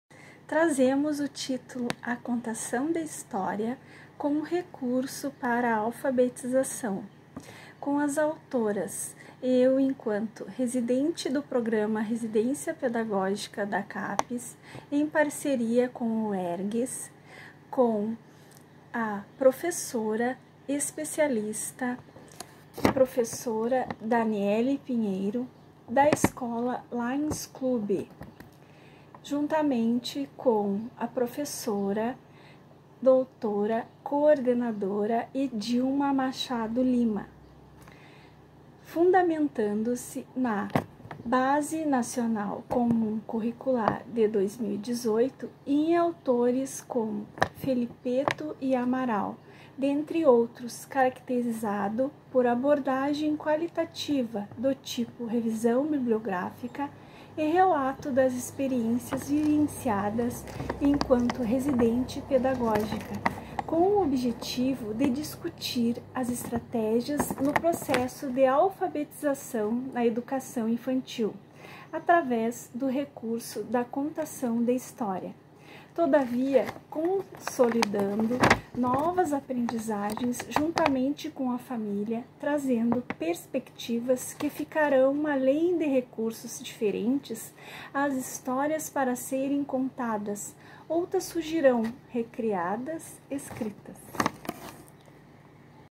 A Contação de História como Recurso para Alfabetização
10o Siepex – Resumo expandido - Graduação – Ciências Humanas – Outros
Silvana Ribeiro Gonçalves,   Edilma Machado de Lima (UERGS) e Dâniele Duarte Pinheiro (Escola LC)
10o Siepex: Conexão Ciência